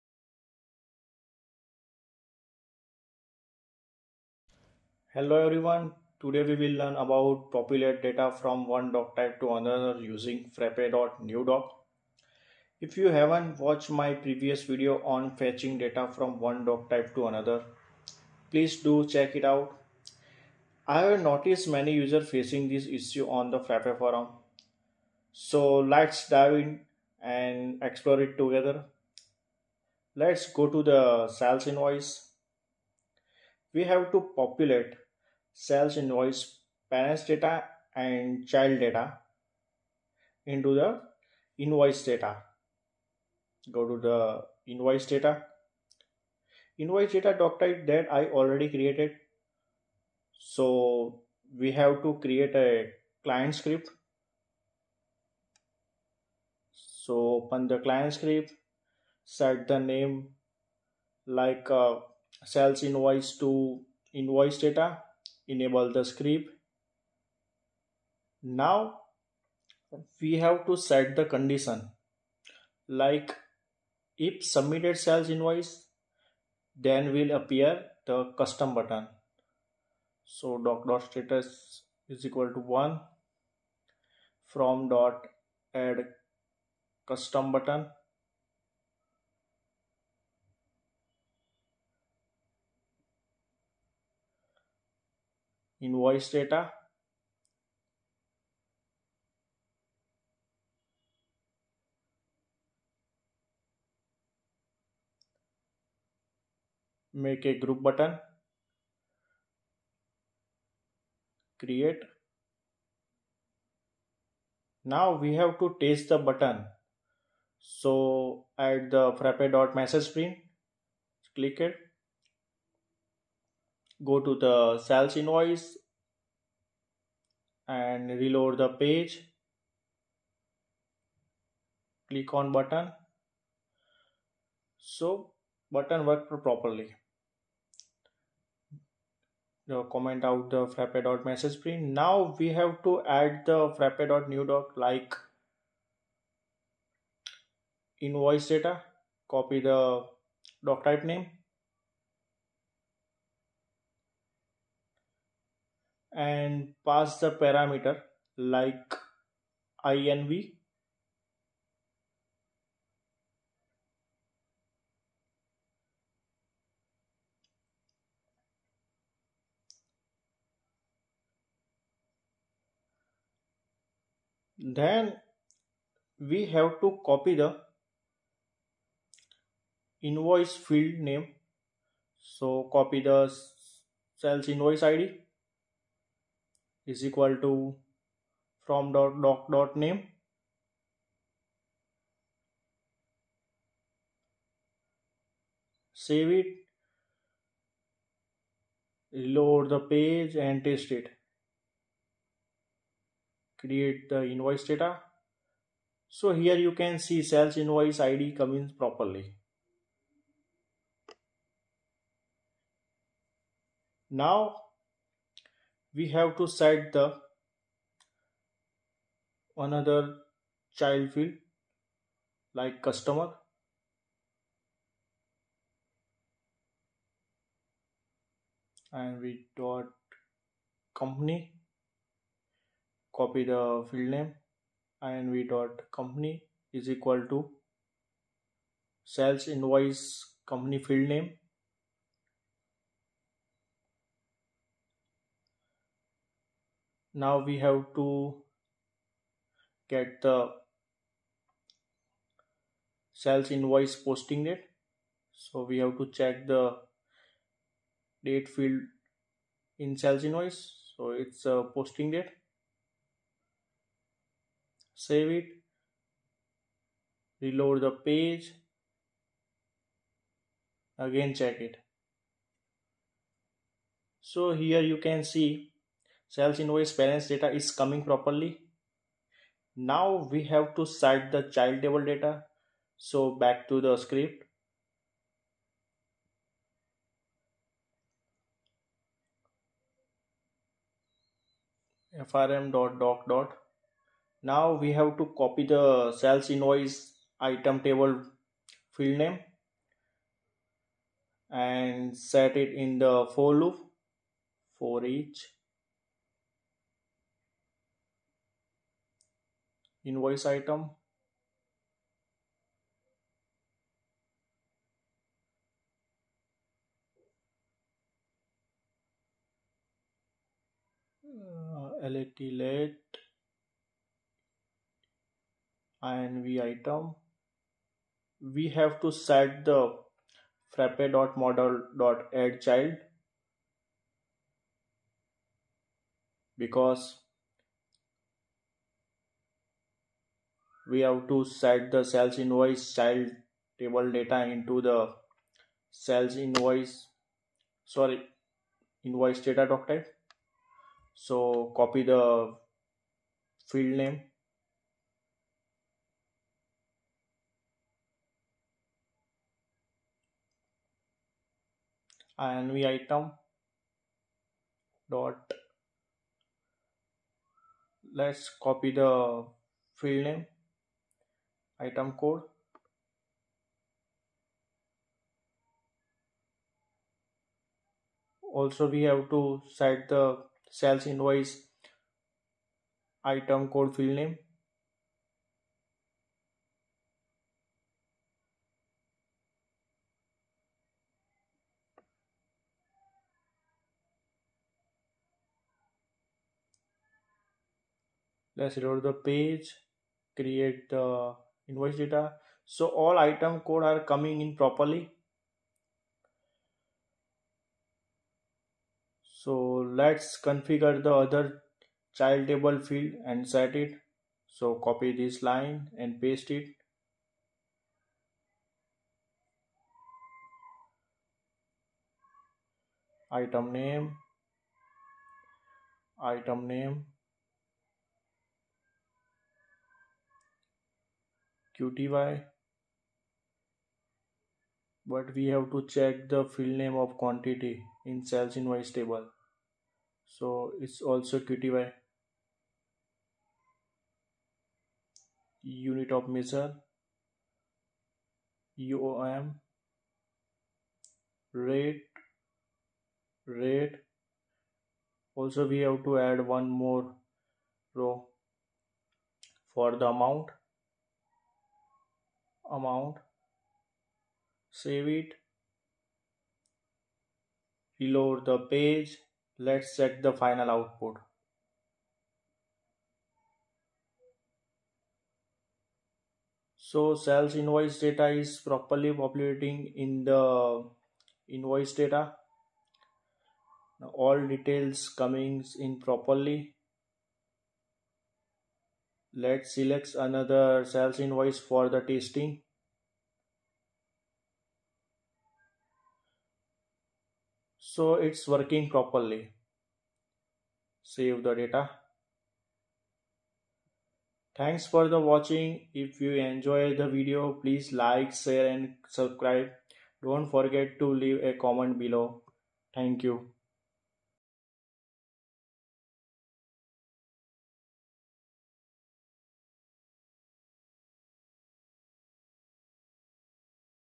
Populate Data From One DocType To Another Using frappe.new_doc | ERPNext
We're tackling a common issue: transferring data between different doctypes using frappe.new_doc(). If you're new to ERPNext or struggling with this process, you're in the right place.

In this video, we'll simplify the process of moving data from one doctype to another. If you're still getting the hang of ERPNext, don't worry - we'll break it down step by step.

If you missed our previous video on fetching data between doctypes, be sure to catch up on that first for a solid foundation.

Let's address this common confusion together and find a simple, permanent solution.